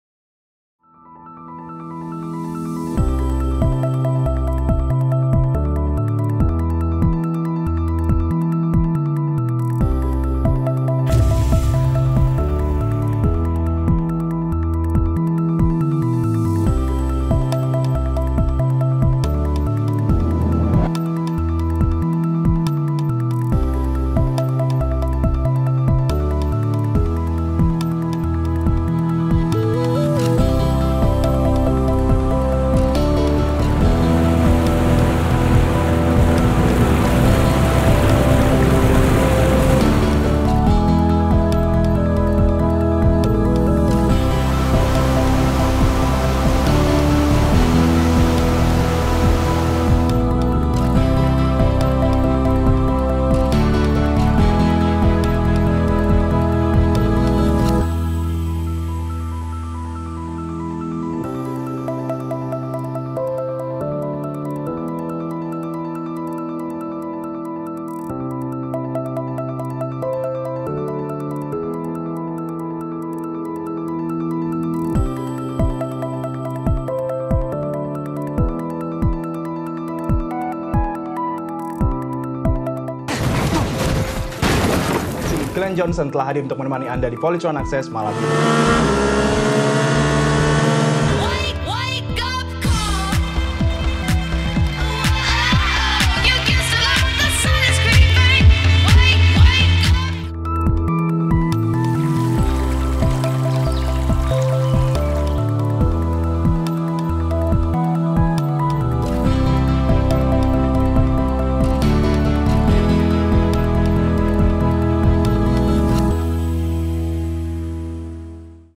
Polytron Smart Soundbar PHT 250 /SB, Nikmati Hiburan Tanpa Batas!
Polytron PHT 250/SB sebuah inovasi baru dengan kecanggihan teknologi kekinian yang dibalut dengan premium design menggunakan tempered glass dan touch panel yang menawan.
Hadir dengan 10 speaker yang hasilkan suara berkualitas dengan konektivitas yang lengkap.
Rasakan pengalaman mendengarkan dengan mode surround yang memberikan efek suara 3 dimensi seperti mengelilingi di segala sisi juga hasilkan suara yang lebih realistis.
Memiliki pengaturan digital menggunakan Aplikasi Polytron Audio Connect, dan makin canggih dengan smart mode yang membuat televisi jenis apapun seolah menjadi smart tv dengan free langganan Mola untuk menikmati tayangan hiburan yang diinginkan.